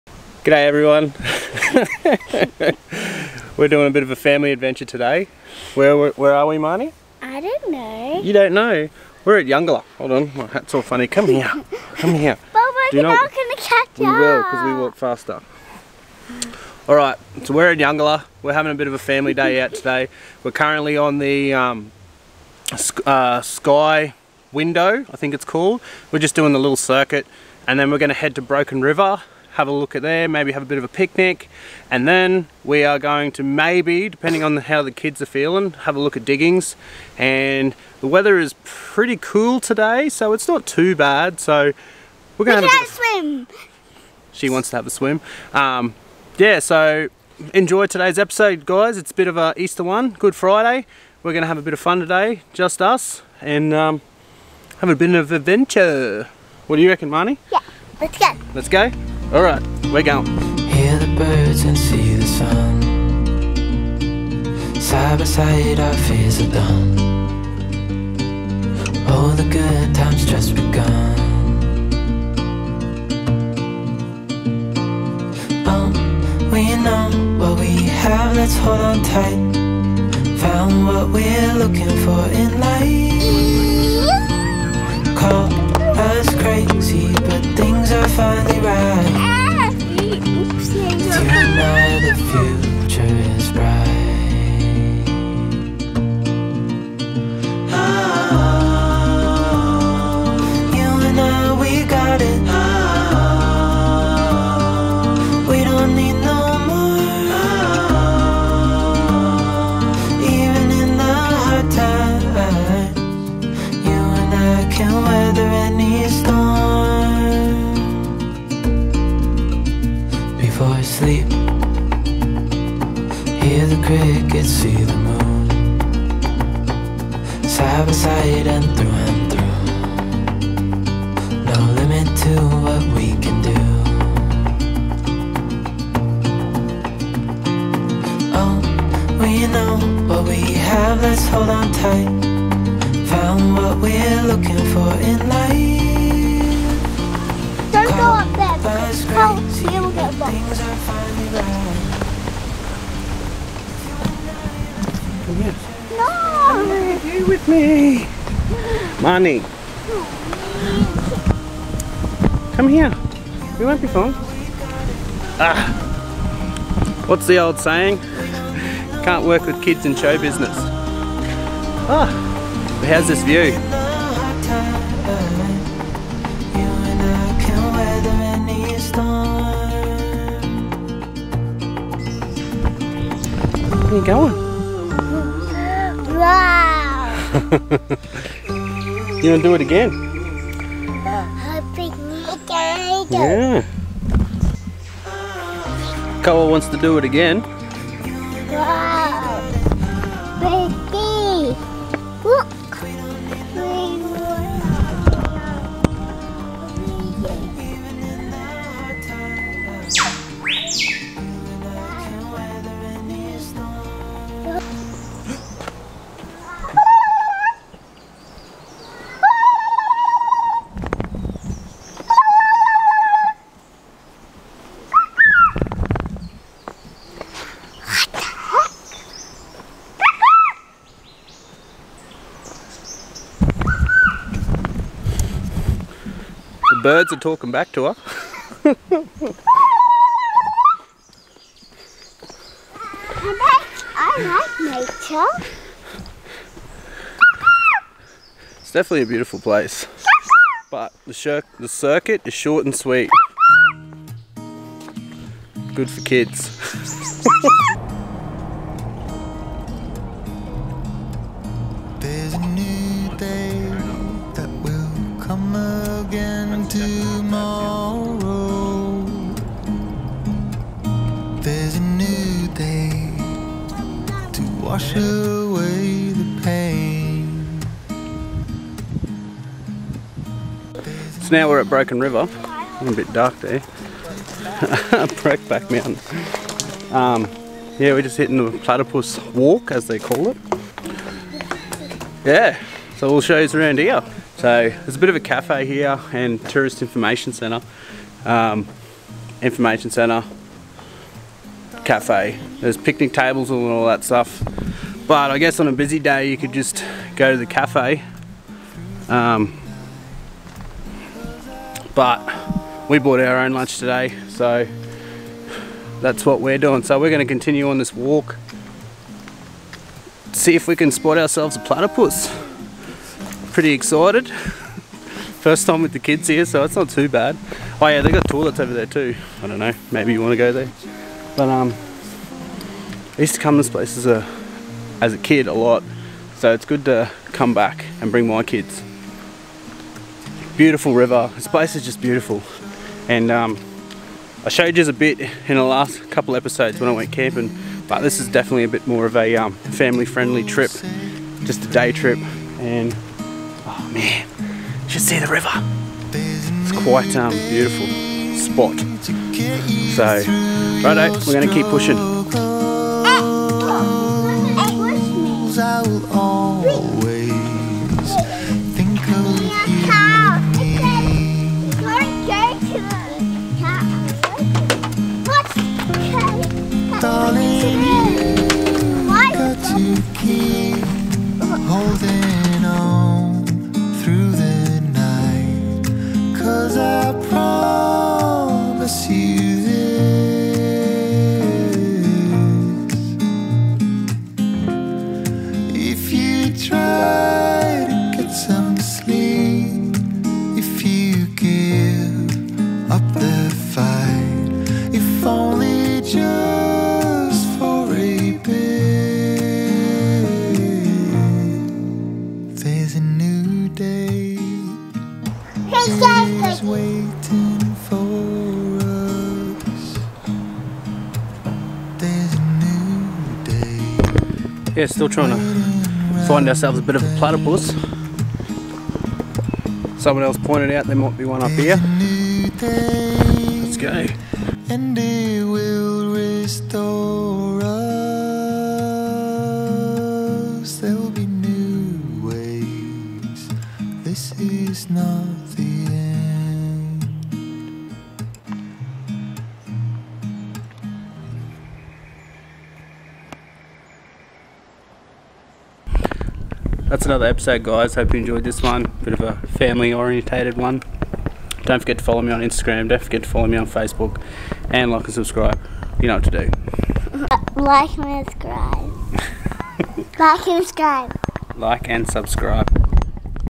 BEST FAMILY SPOT EUNGELLA MACKAY LANDCRUISER EUNGELLA #
Its good Friday an we packed up the car an taking the kids to Eungella for the first time. we check out the sky window track an have a lovely family ADVNCHUR so i hope you guys enjoy this one

Get into it an " HAVE A GO"